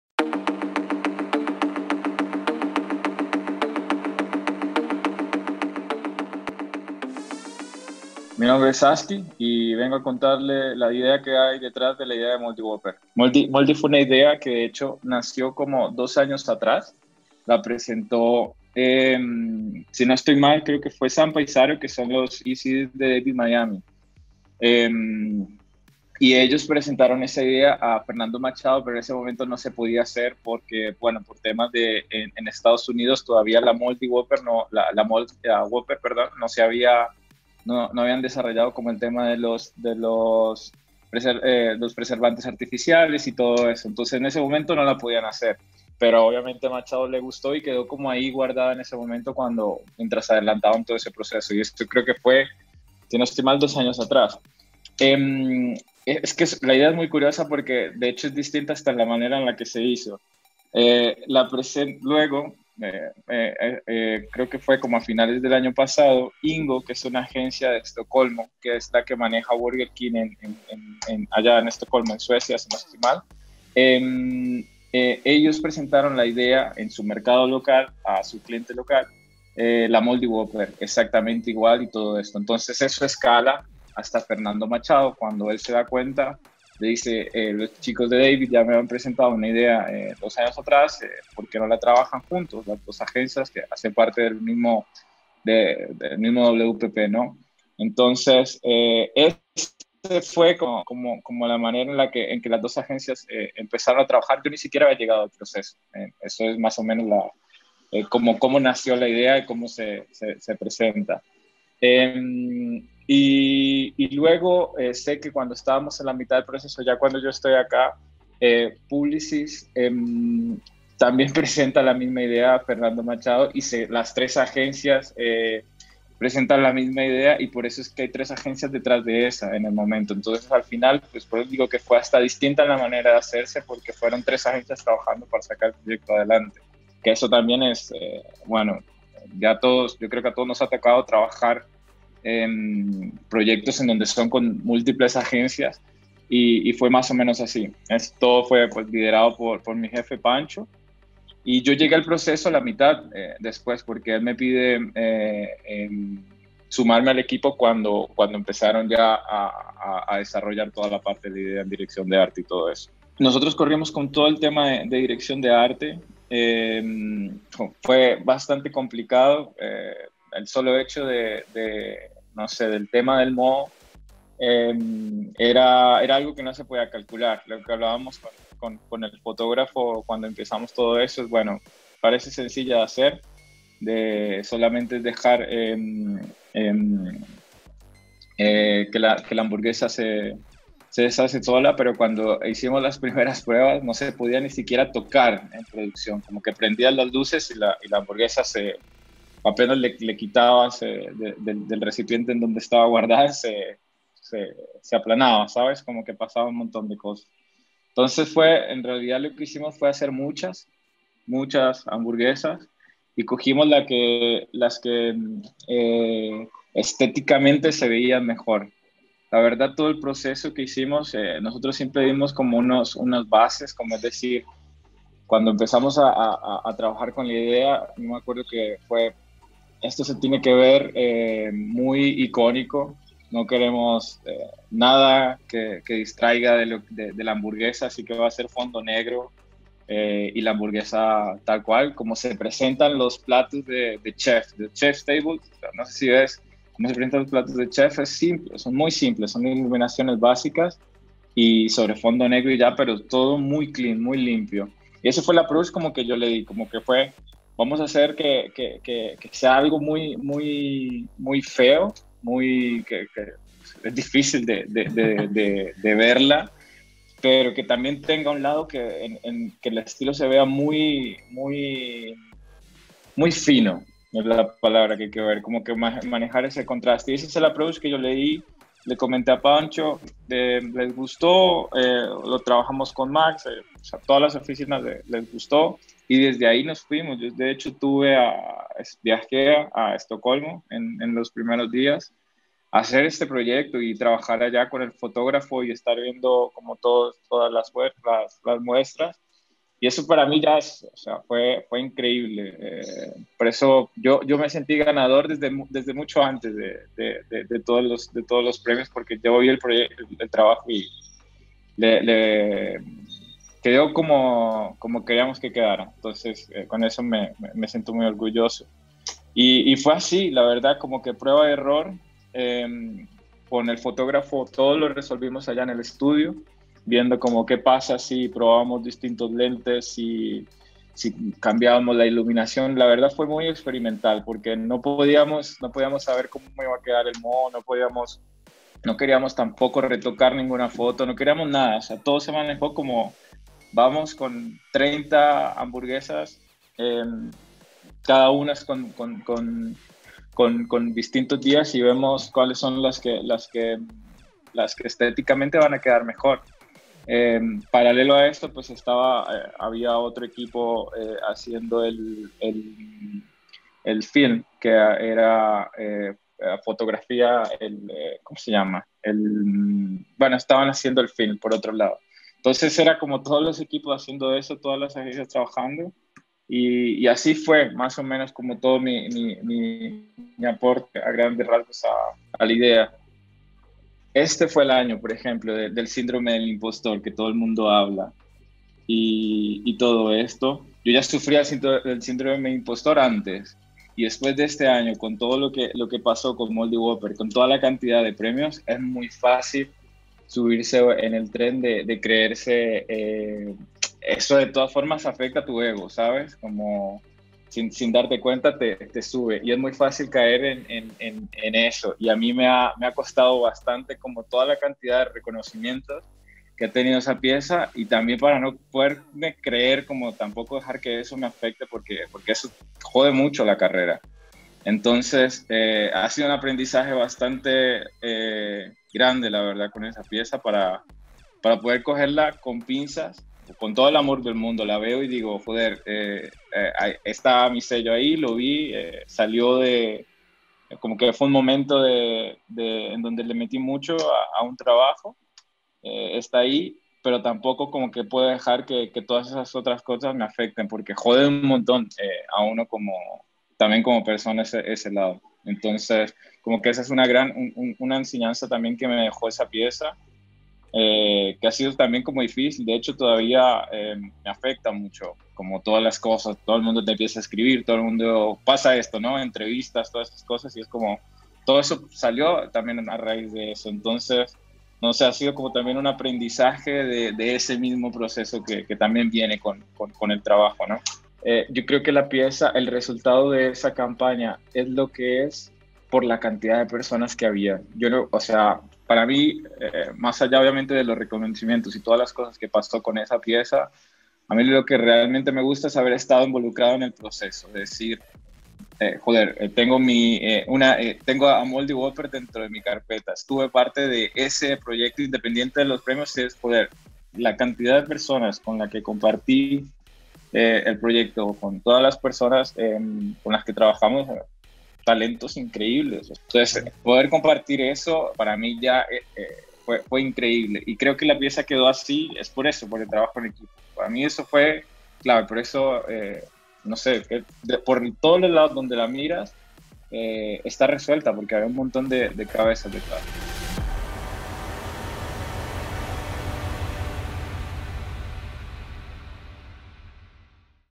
MOLDY WHOPPER | Camilo Jiménez (Sazky), Sr. Art Director DAVID Miami
Burger King muestra en 35 días cómo realmente se ve una hamburguesa libre de conservantes artificiales, una apuesta arriesgada de tres agencias.